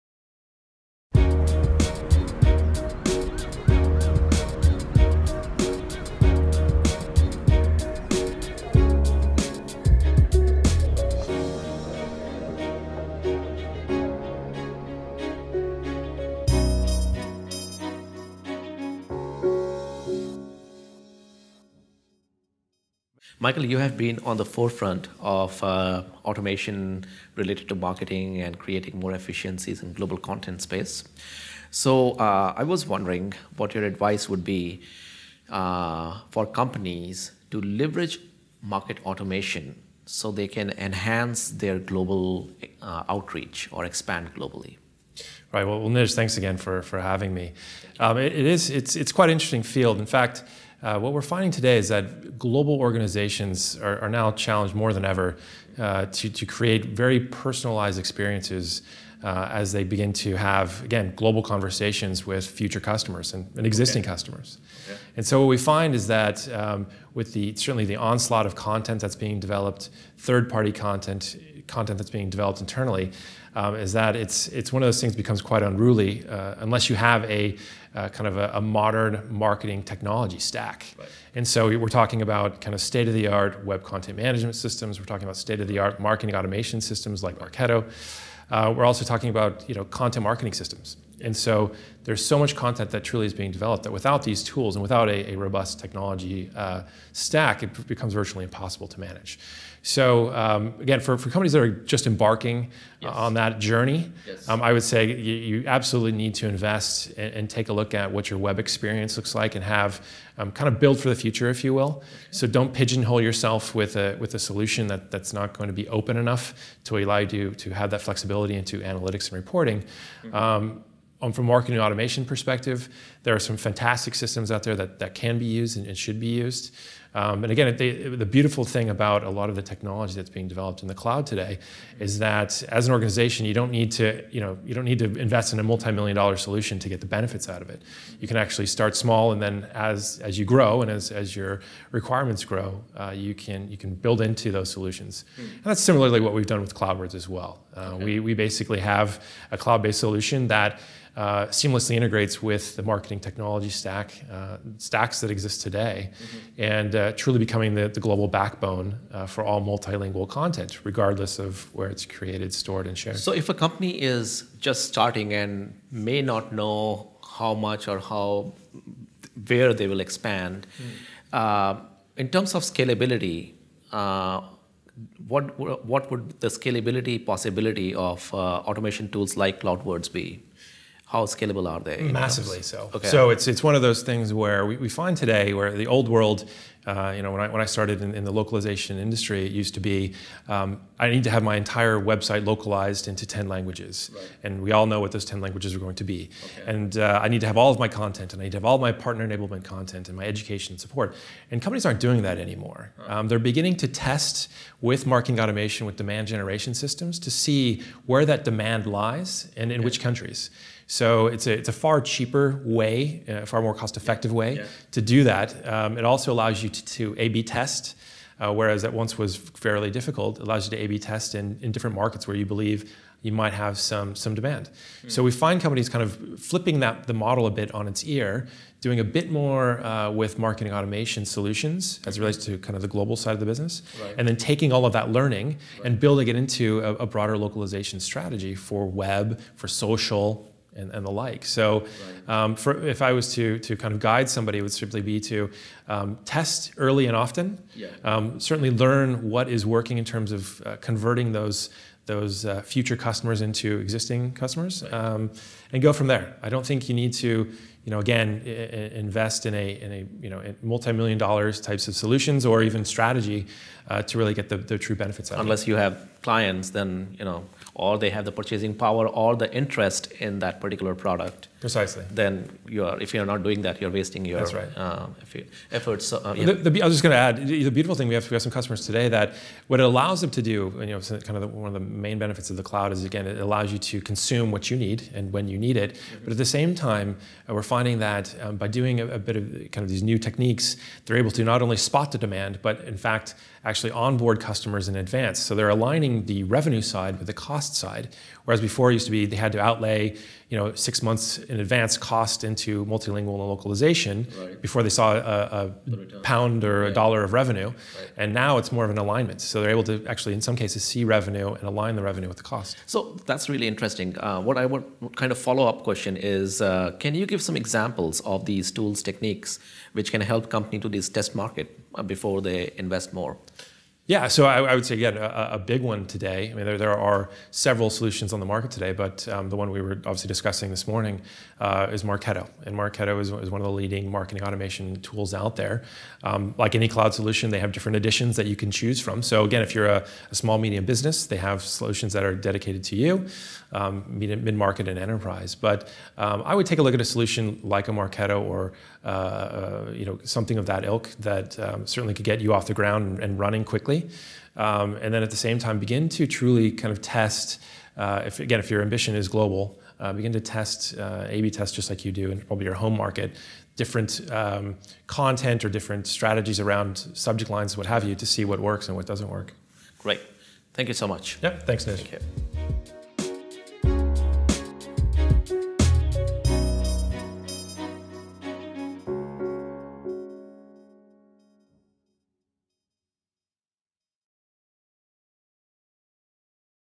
Leveraging Marketing Automation for Global Marketing - Michael Meinhardt, CCO Cloudwords
Michael discusses how the pace and quantity of content creation begs the question of automation. Marketing automation is necessary for achieving scalability and effective deployment of global content. However, companies do not need to invest in mulit million dollar automation solutions to manage their global content. Companies can use various emerging cloud-based automation technologies that are both cost effective and scalable. Learn more in this interview about marketing automation technologies.